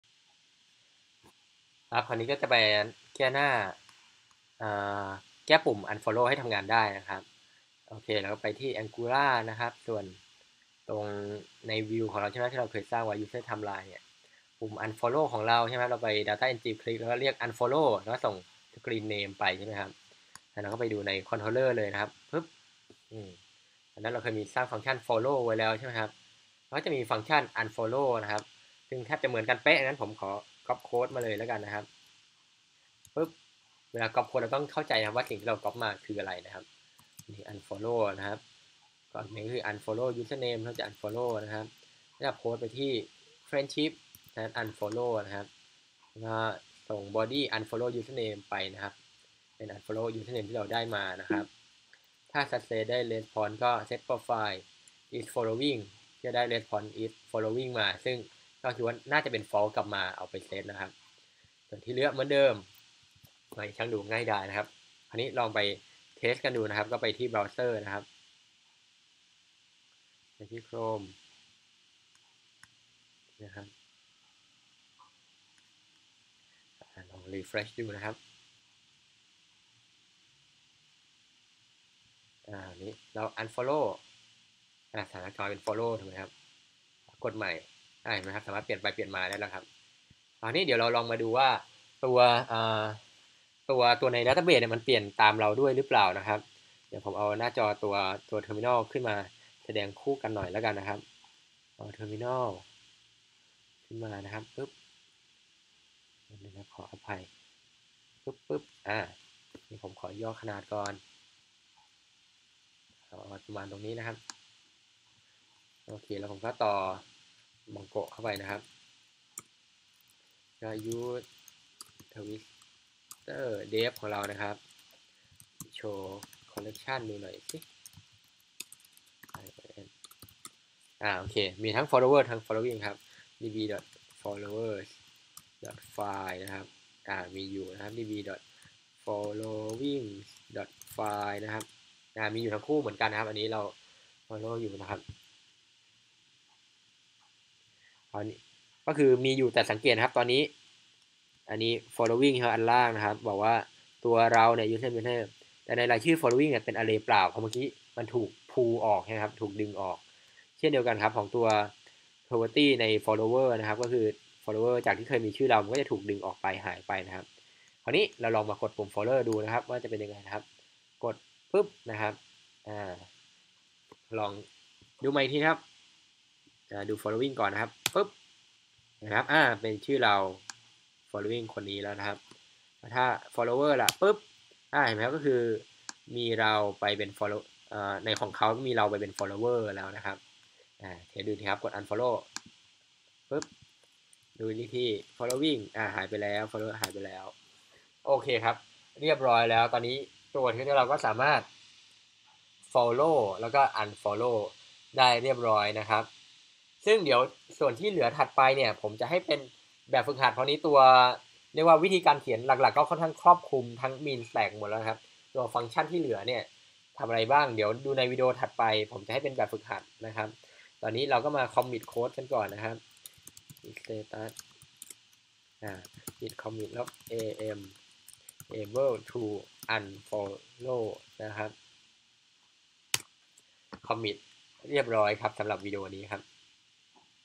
[11-46] แก้ไขหน้า usertimeline ให้รองรับการกดปุ่ม unfollow
แก้ไขหน้า usertimeline ให้ตรวจสอบสถานะ friendship และรองรับการกดปุ่ม unfollow